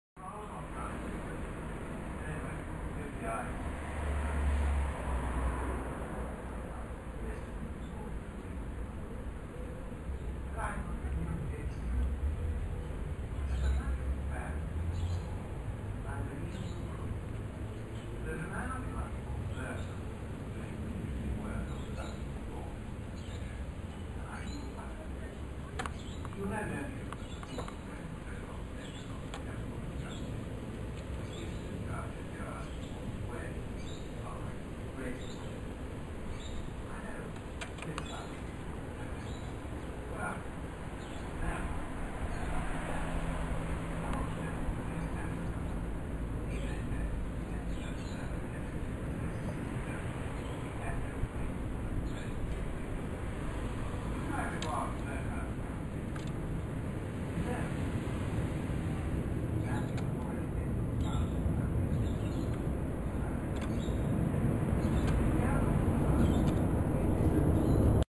Jay on my balcony
European jay - one of four that come regularly to my balcony for food.